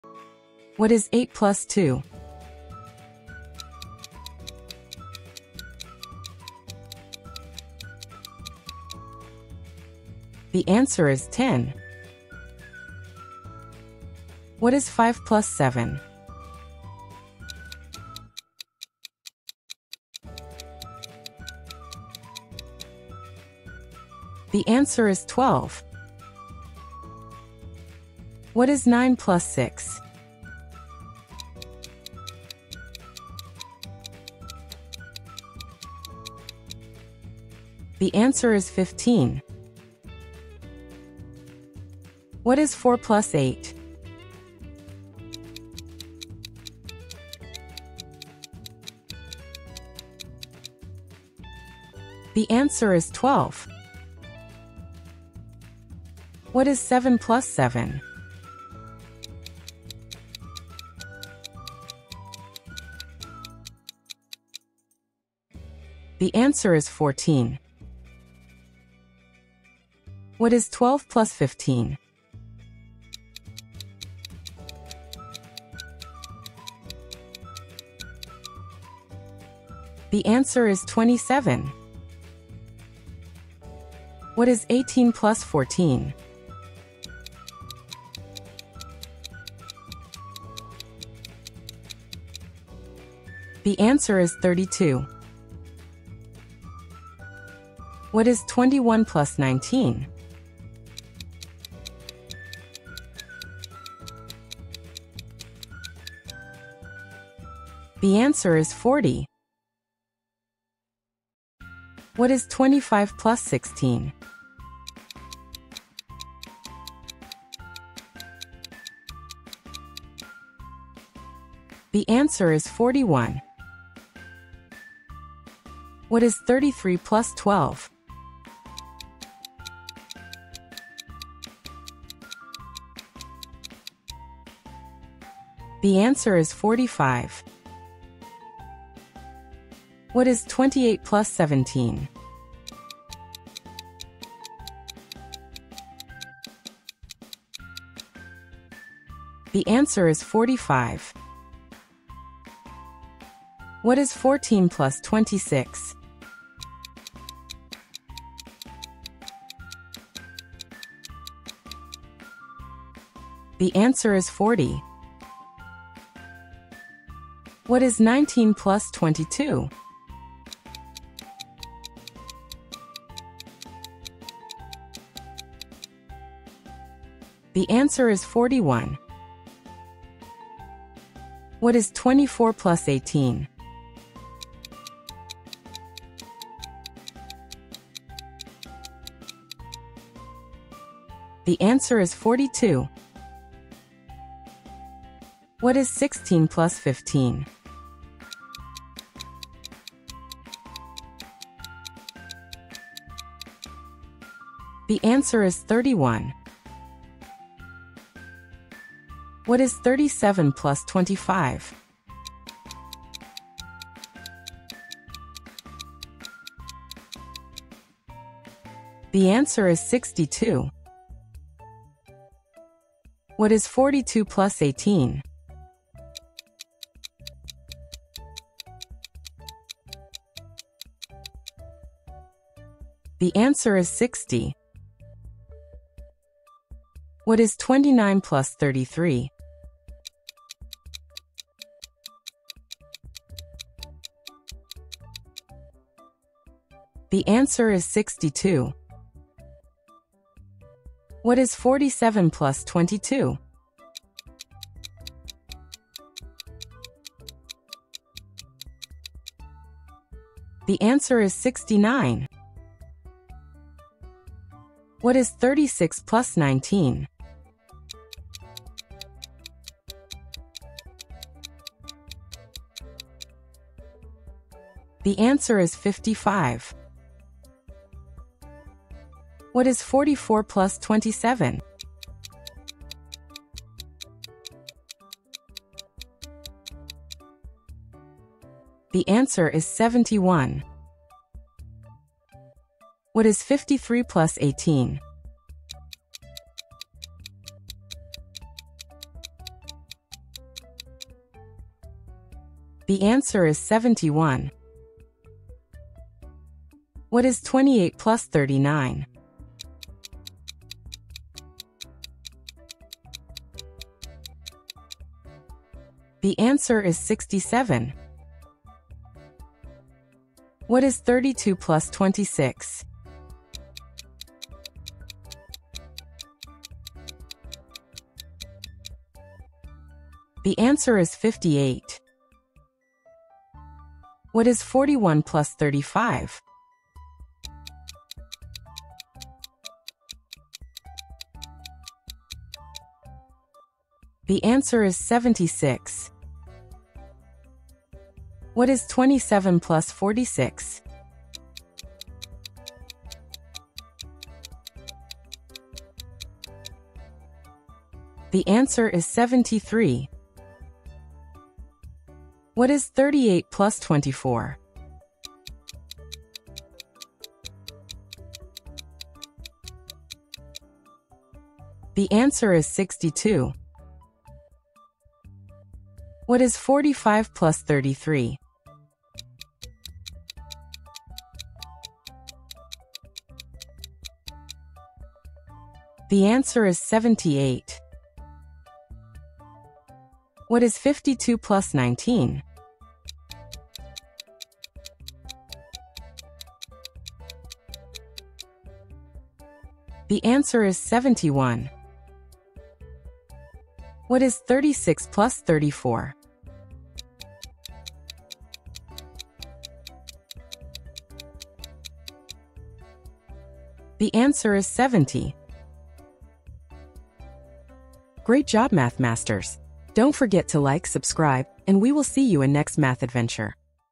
🌟 Learn Addition Numbers | Grade 2 Math Practice for Kids
Make math exciting! 🧮 This Grade 2 addition video offers step-by-step practice, colorful visuals, and simple challenges that boost confidence while making learning fun and engaging.